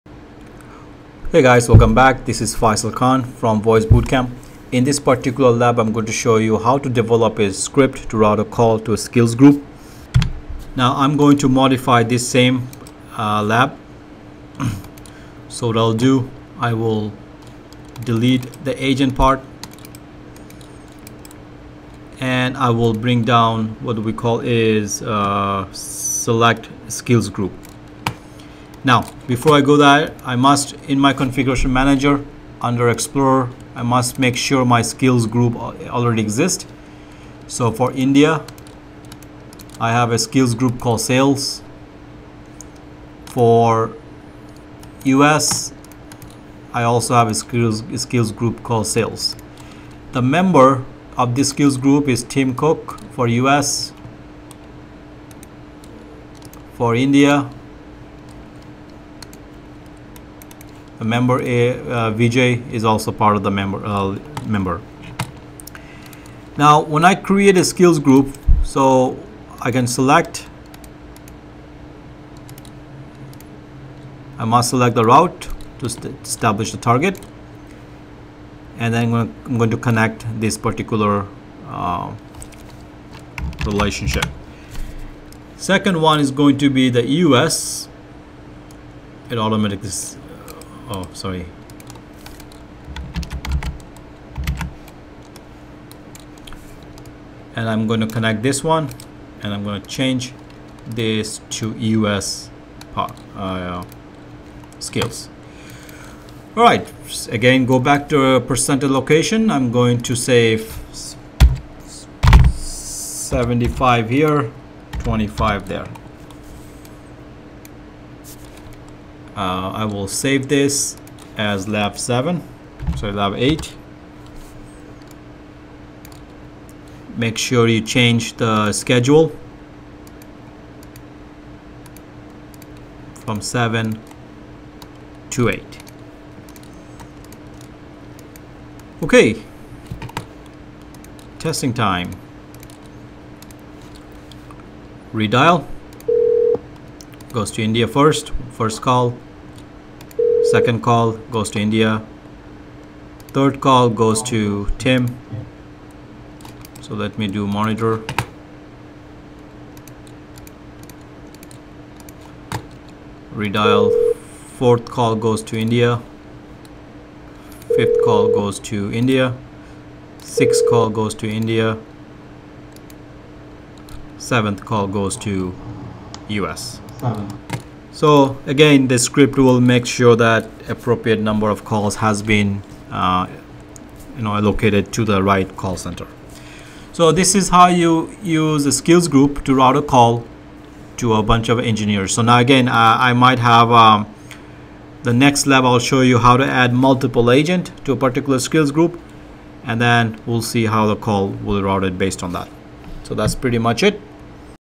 UCCE Scripting Lab 8 Develop Script to route calls to Skills Group
by Faisal H khan

Cisco UCCE 10.5 Advanced Scripting Self Study Kit